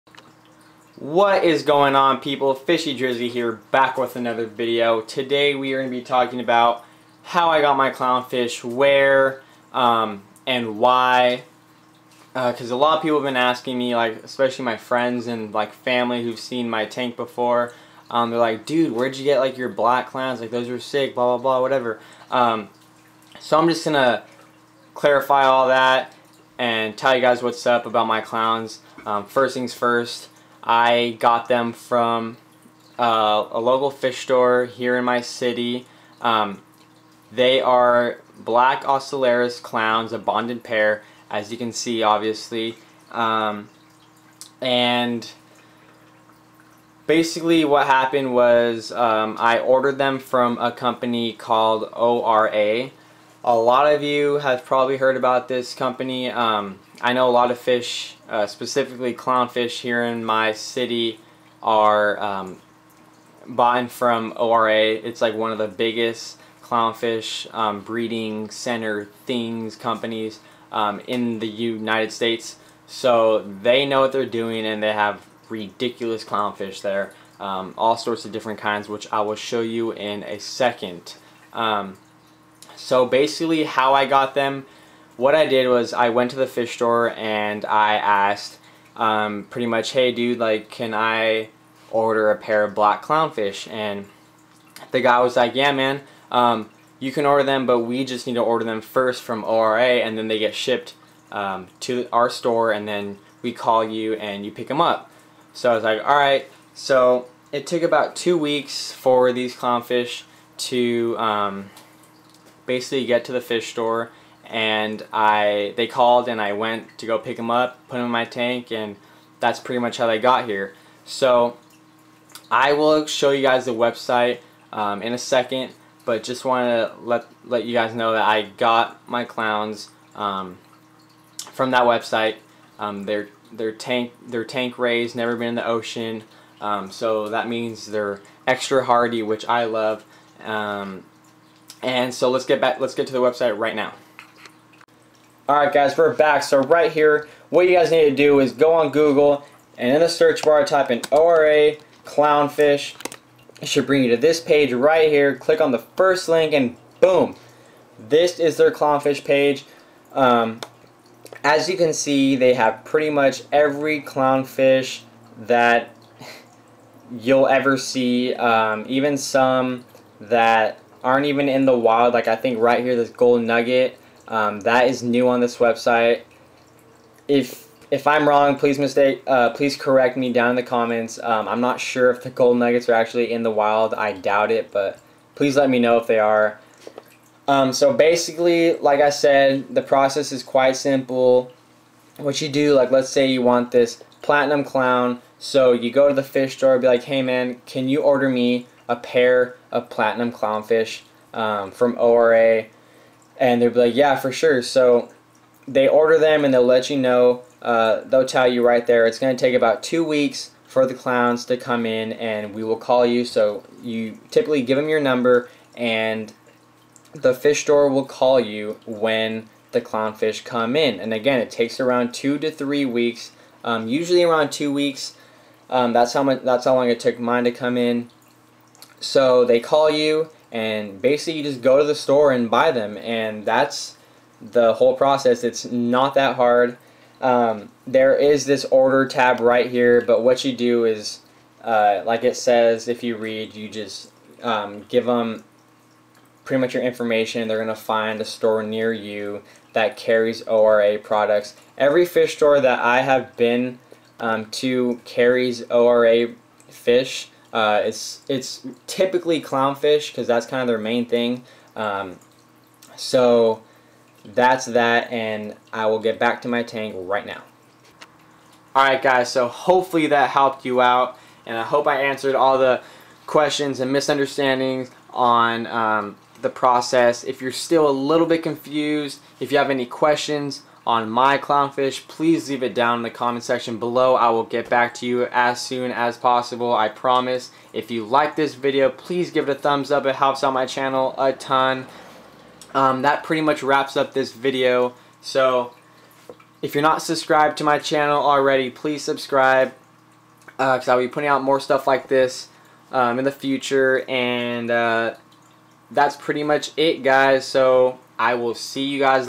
How to get Clownfish from ORA
Hey all, todays video is going to show how to order clownfish from ORA. It's a very simple process and anybody can do it! They have every clownfish you can think of plus more and they are ALL great quality fish. I have had mine for 2 years and they are still going strong!

Thanks!